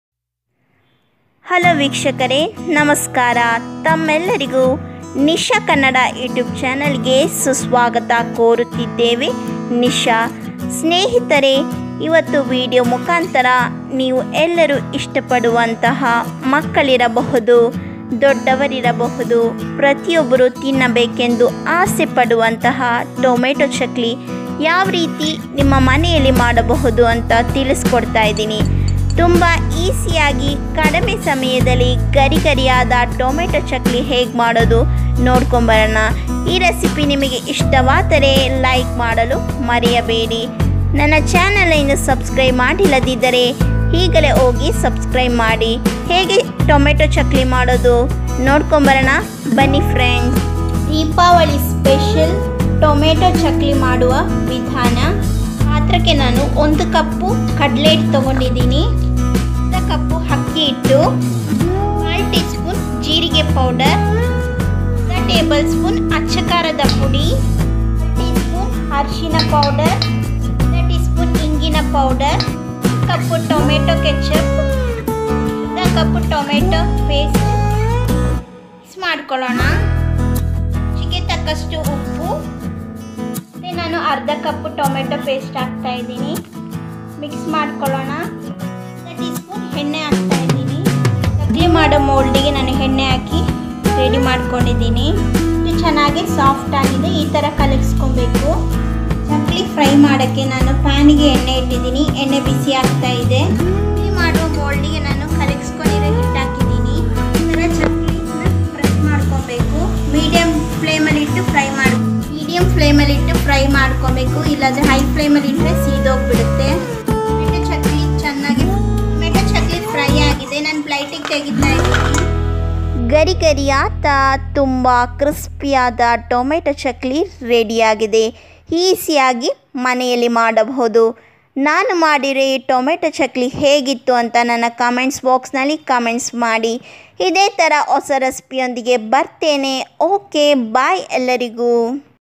EP:94 Diwali Special Snacks Tomato Chakli /Tomato Murukku /Chakli recipe just 5 minutes
Hi friends Diwali Special Snacks Tomato Chakli /Tomato Murukku recipe /Tomato Chakli just in 5 minutes